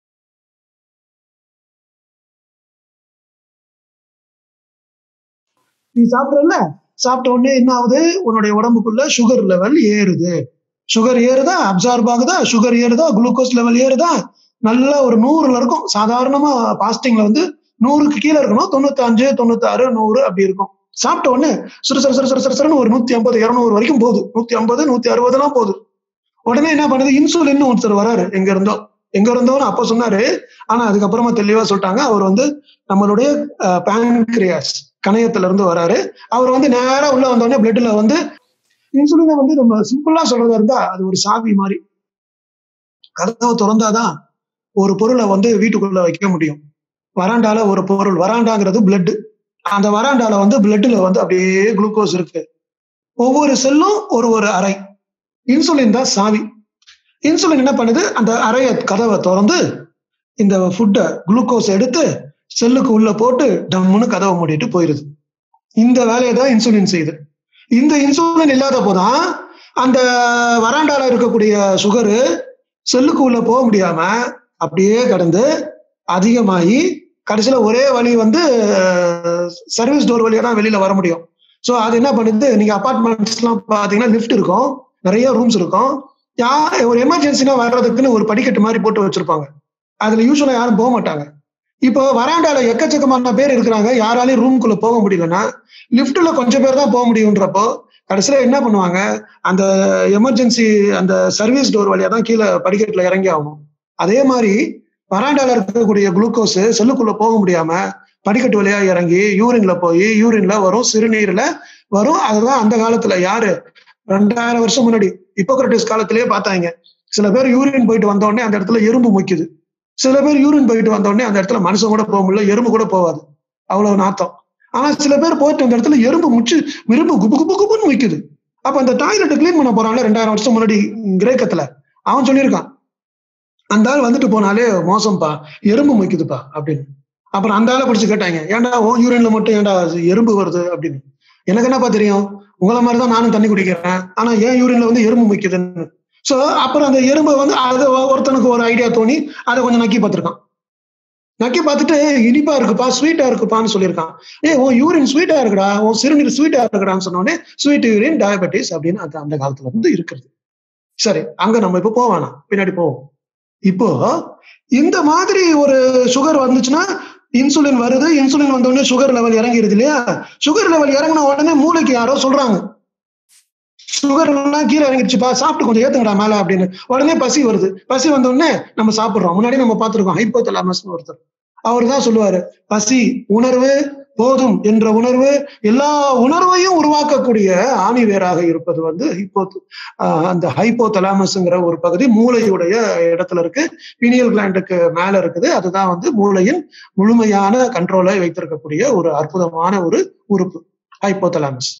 MMSTVL19AAAF | ஆன்மீக அறிவியல் | இன்சுலின் உயிரணுவின் சாவி போன்றது பேராசிரியர் | Dr செம்மல்
கணையநீரிற்கும் (Insulin) சர்க்கரை சத்திற்கும் இடையே உள்ள தொடர்பு என்ன ?
சர்க்கரை நோயை ஆதிகாலத்தில் எவ்வாறு அறிந்தனர் ?

இறைவனின் அற்புத படைப்பியல் பற்றி
பேராசிரியர் டாக்டர் செம்மல் வழங்கும்
ஆன்மீக அறிவியல் உரை - பகுதி 2